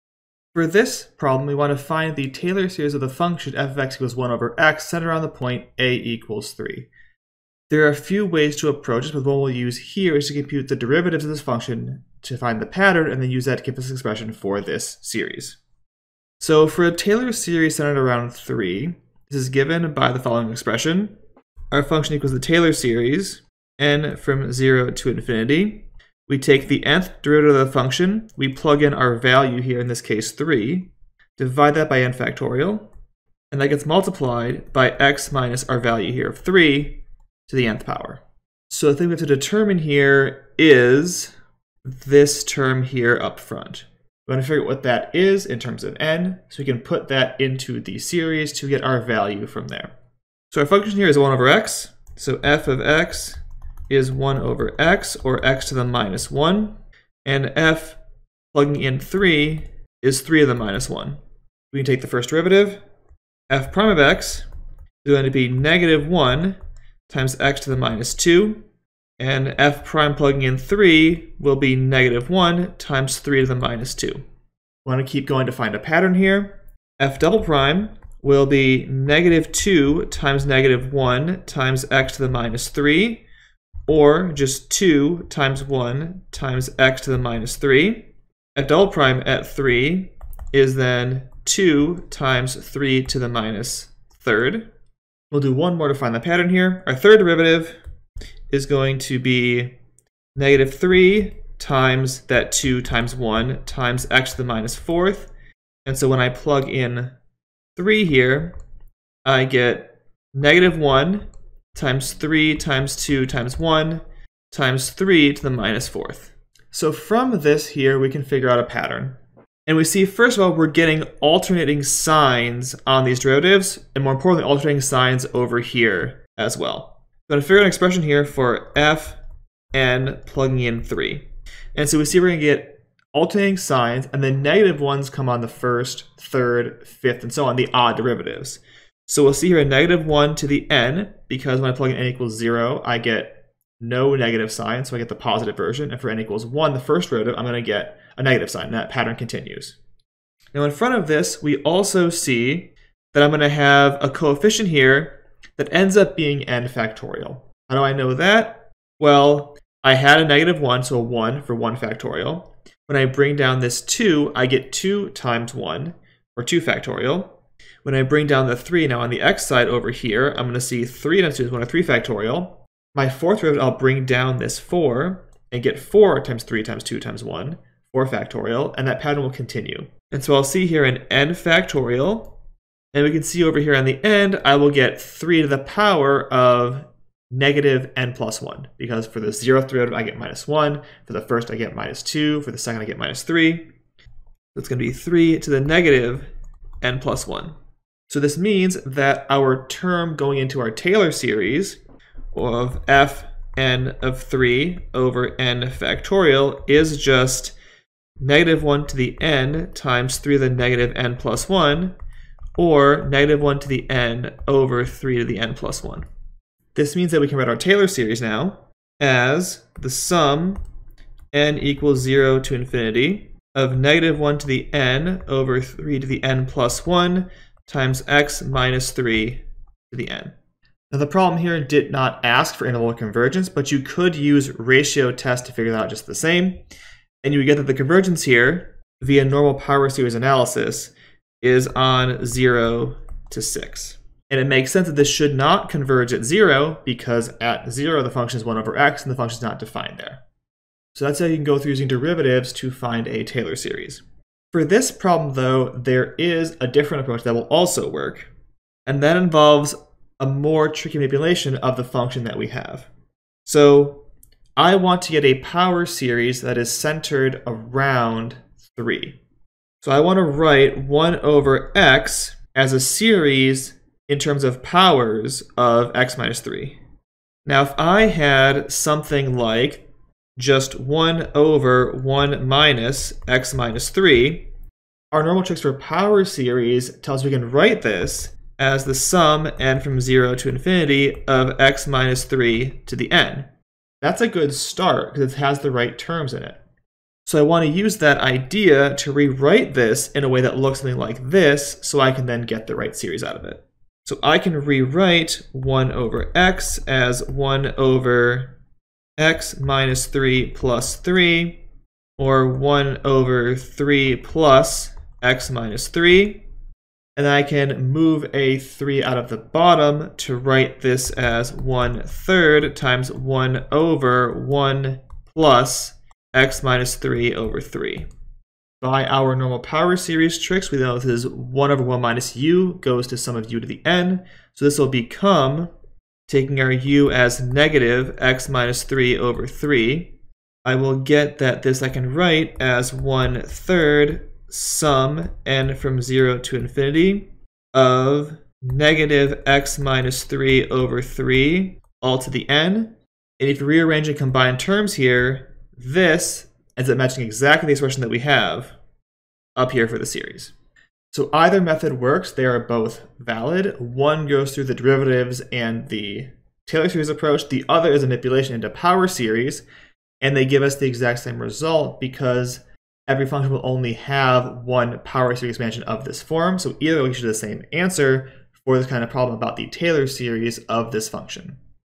Calculus 2 - Taylor Series - Practice Problem
Example working out a known formula (geometric series) using a Taylor Series approach.